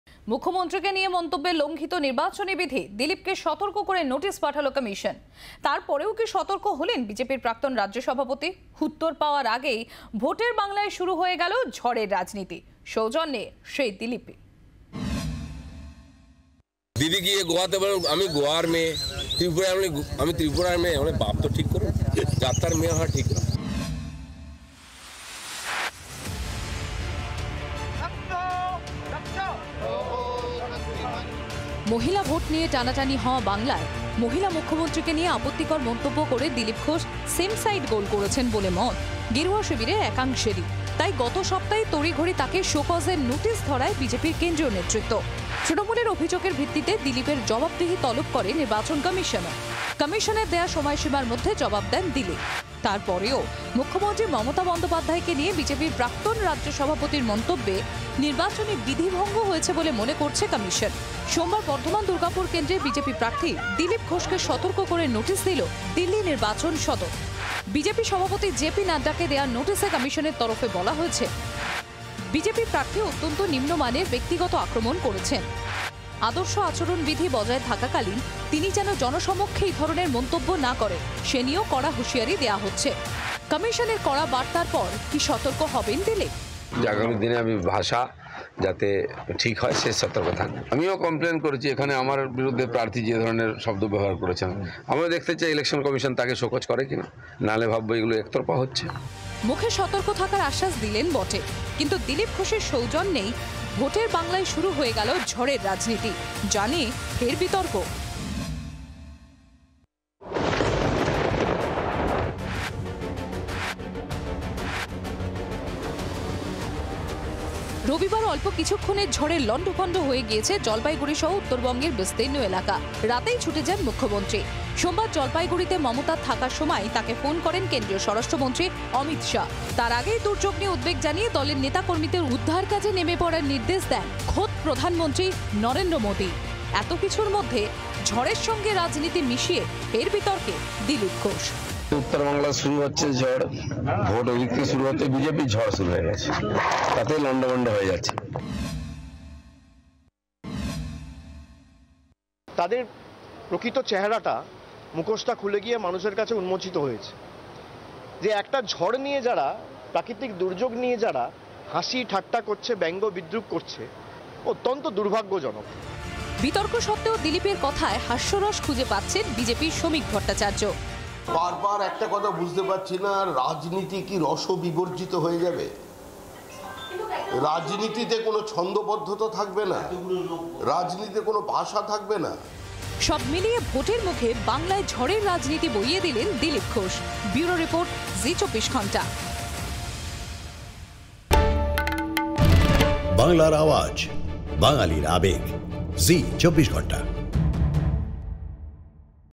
Dilip Ghosh: দুর্যোগে রাজনীতি মিশিয়ে ফের বিতর্কে দিলীপ ঘোষ | Zee 24 Ghanta
Dilip Ghosh: দুর্যোগে রাজনীতি মিশিয়ে ফের বিতর্কে দিলীপ ঘোষ, জেনে নিন এই মুহূর্তের সেরা আপডেট...